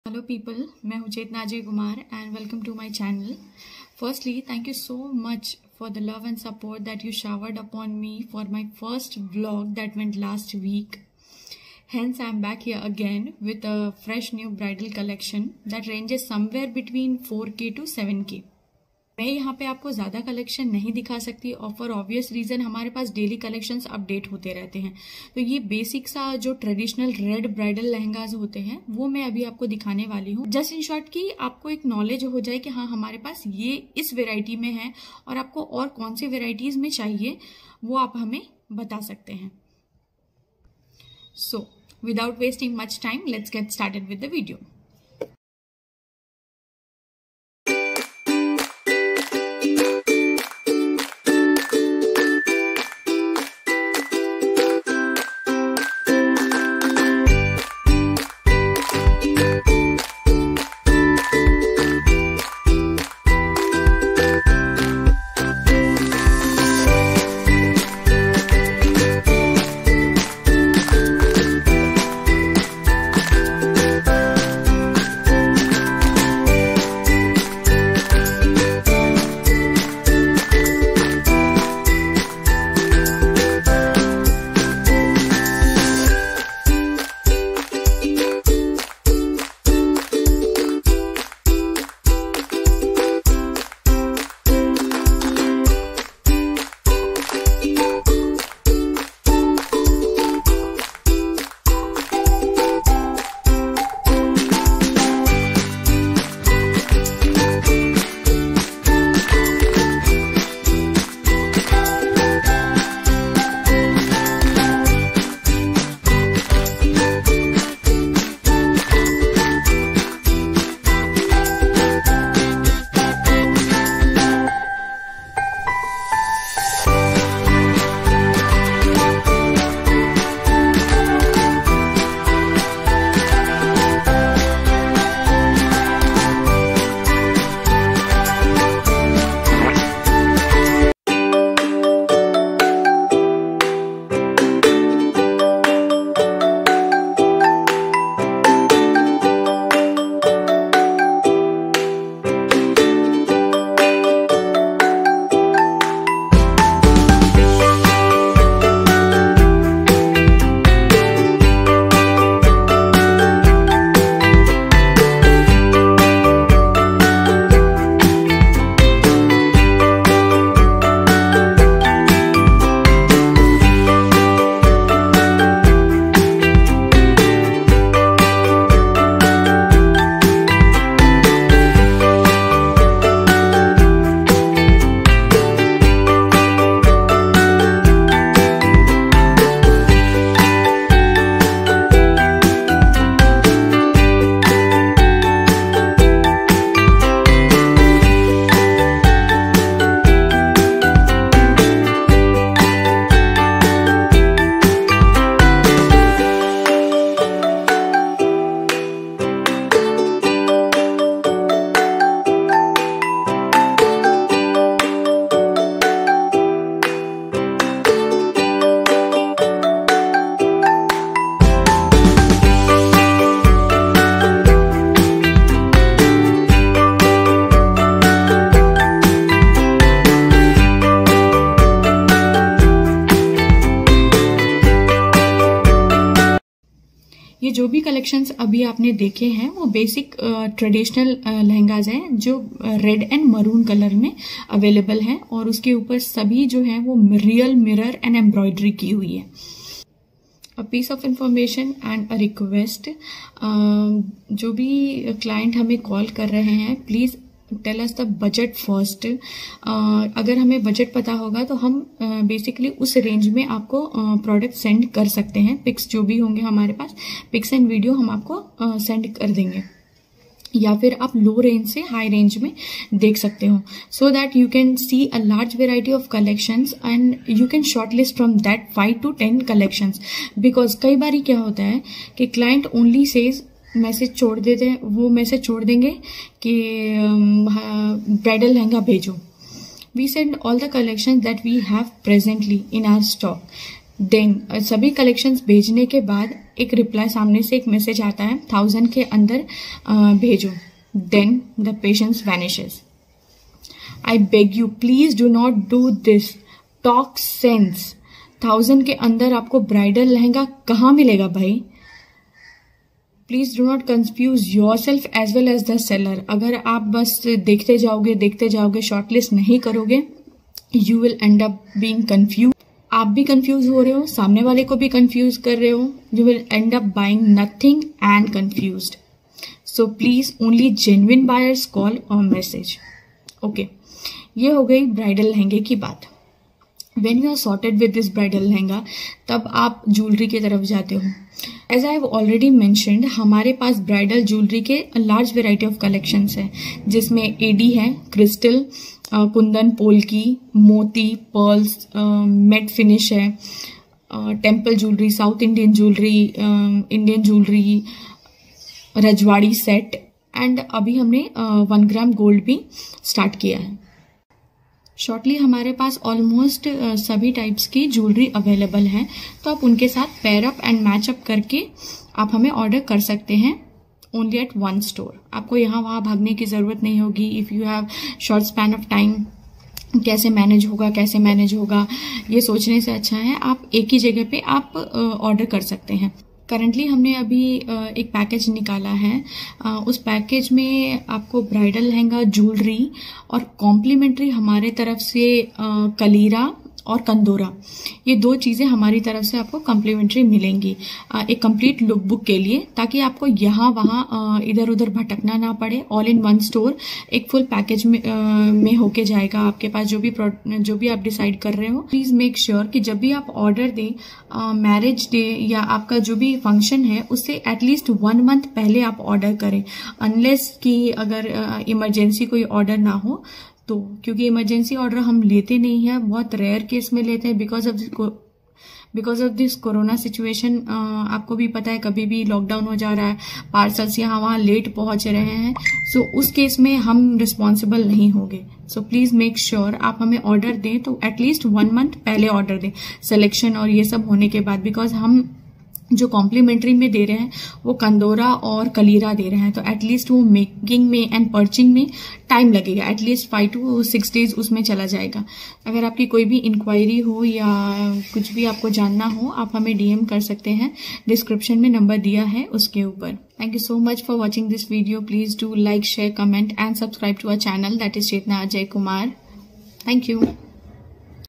Rs.1000 me bridal lehenga 😳😳😟
Hi people,

I am chetnajay kumar. This video is regarding the new arrival of bridal lehenga and jewellery.

fb page "Dressingpoint"

Spread Love ❣️